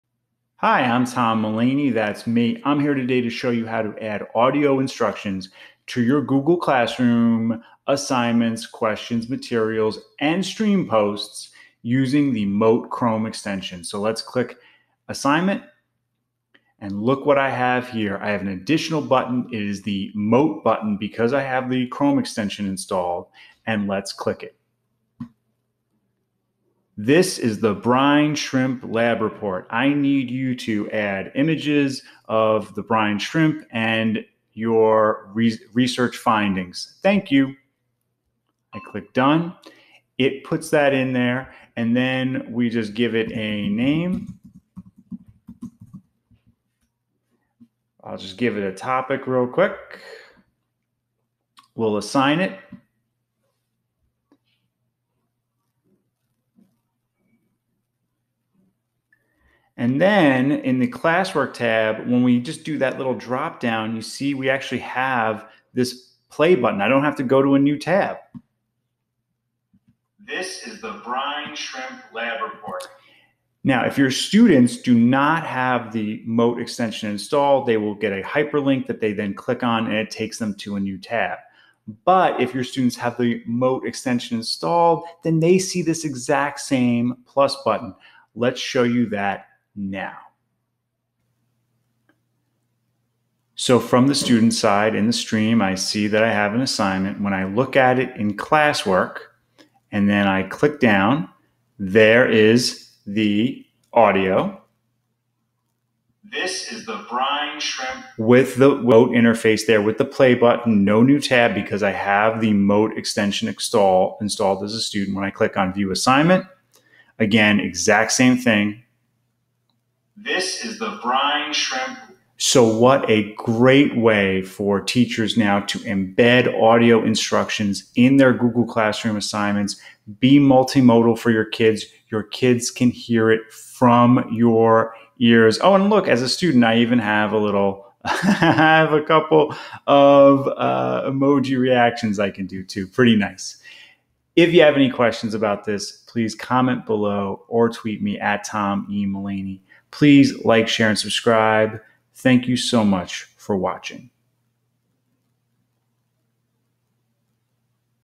Add Audio Instructions to Google Classroom Assignments with Mote
Use the Mote Google Chrome extension to add audio to any Google Classroom assignment, question, material, or Stream post.